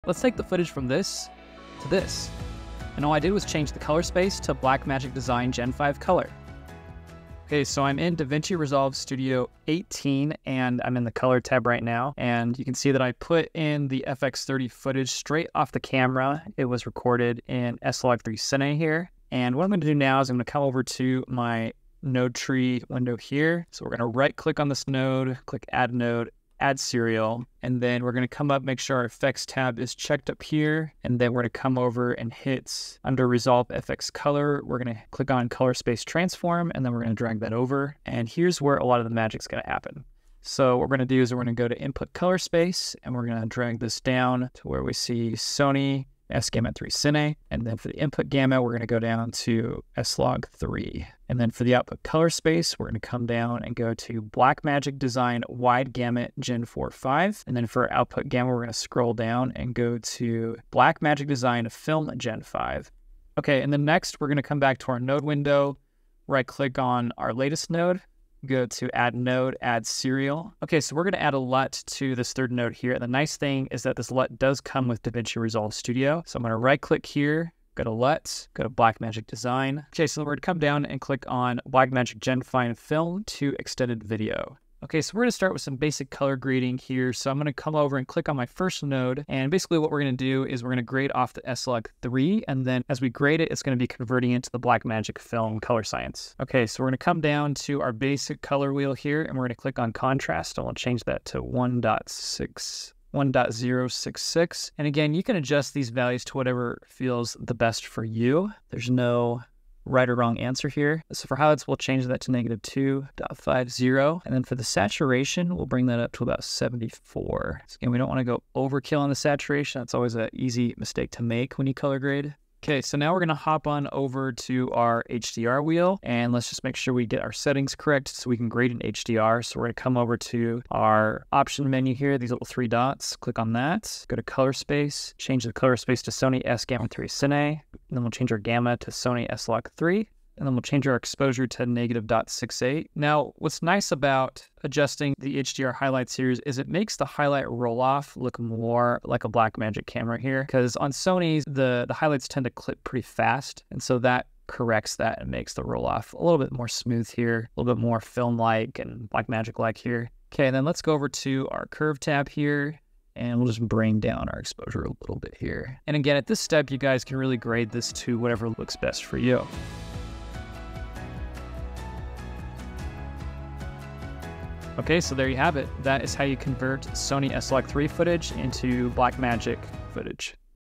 Convert S-Log3 Into Blackmagic Gen 5 Color Using Color Space Transform In DaVinci Resolve 18
Book Your 1 On 1 DaVinci Resolve Tutoring Session With Me

Explore the world of color grading in DaVinci Resolve Studio 18 with our tutorial. Follow the simple steps to transform SLog3 Cine footage from Sony S-Log3 to the exquisite Blackmagic Design Gen 5 color. Elevate your videos and captivate your audience with this easy-to-follow guide.